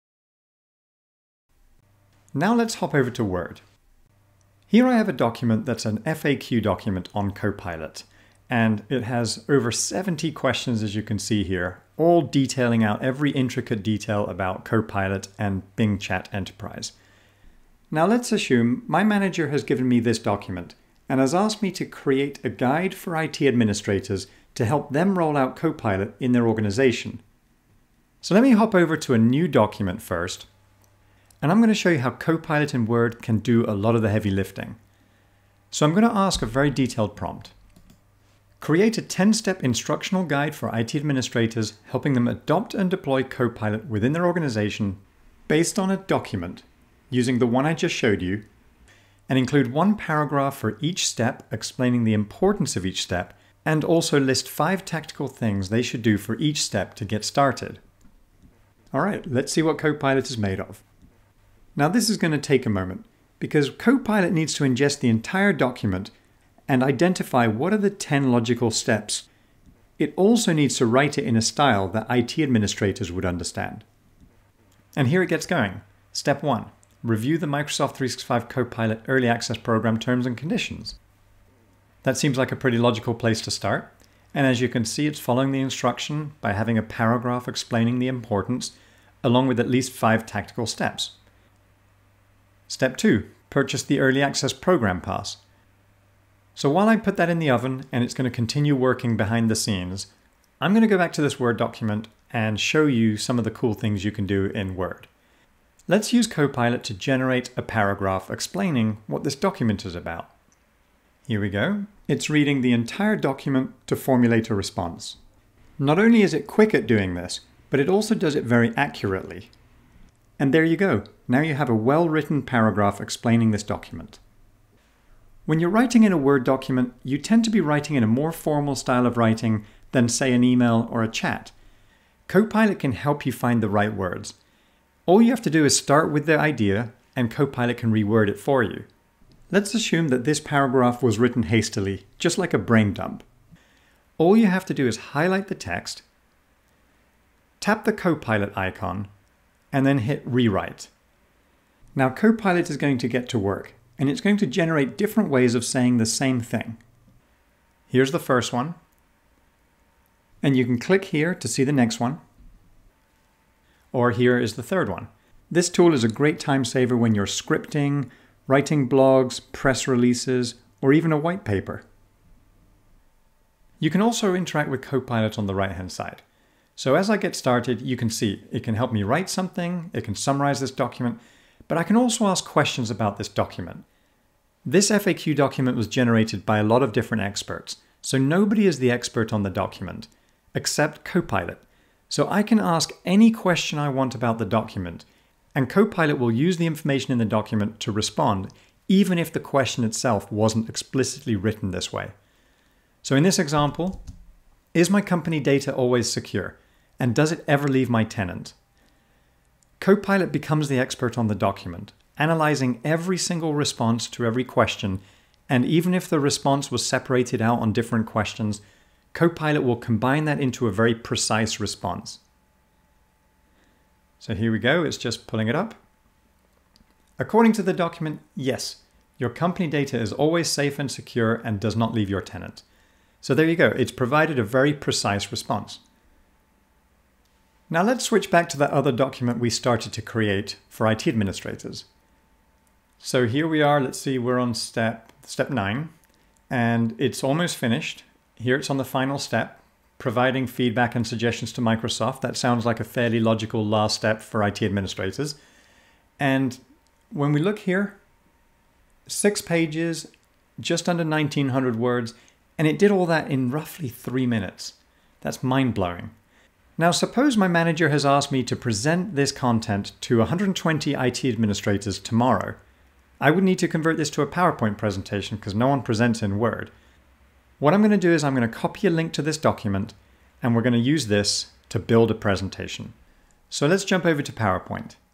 【Copilot for Microsoft 365 デモ動画】Copilot in Word のご紹介
【Copilot for Microsoft 365 デモ動画】 Copilot in Word を使えば、これまでにないやり方でドキュメント作成ができます。原稿のドラフトや再作成、内容の要約や質問などに Copilot を活用しましょう。
▼ AIで一変する、組織と個人の働き方 (日本マイクロソフト 作成)：